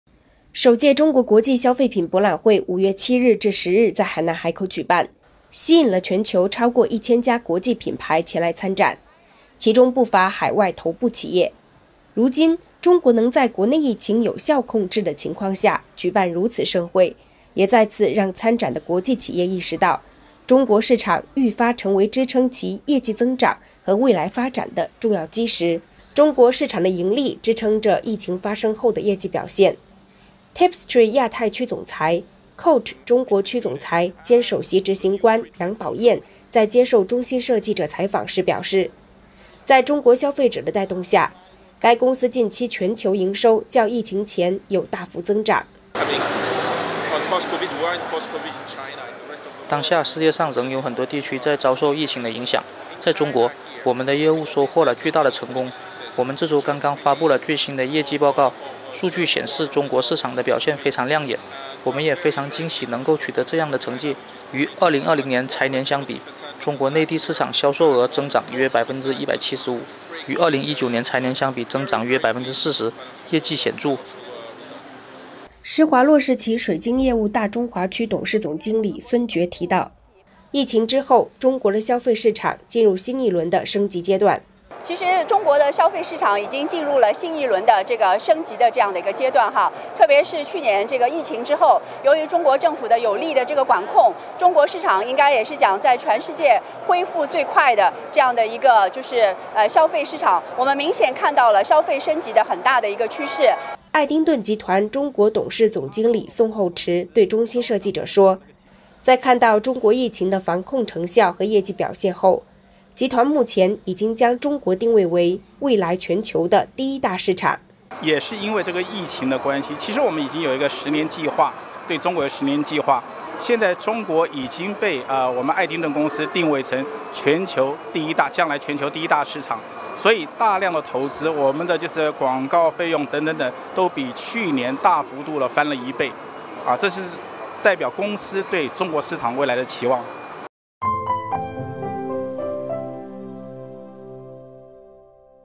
全球品牌看疫后消费市场：中国是业绩增长的重要基石
首届中国国际消费品博览会5月7日至10日在海南海口举办，吸引了全球超过一千家国际品牌前来参展，其中不乏海外头部企业。如今，中国能在国内疫情有效控制的情况下，举办如此盛会，也再次让参展的国际企业意识到，中国市场愈发成为支撑其业绩增长和未来发展的重要“基石”。